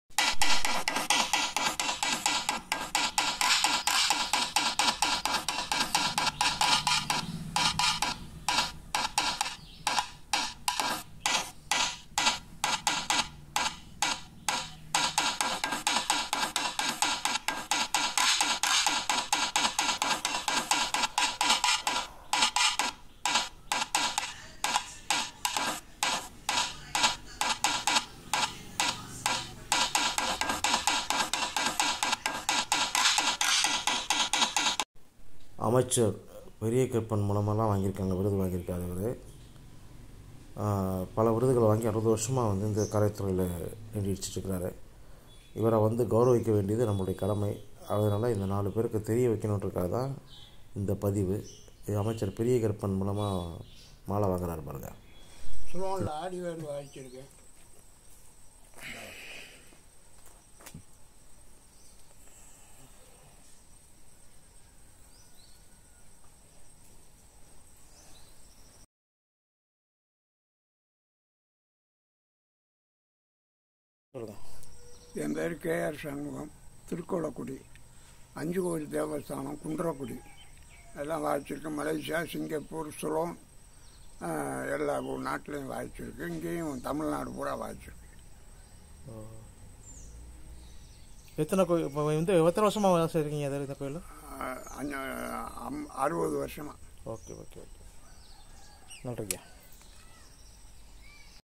Olive Krishna Tv
60 ஆண்டுகளாக தவில் வித்வான் என புகழ்ப்பெற்ற திரு. K. R. Shanmugam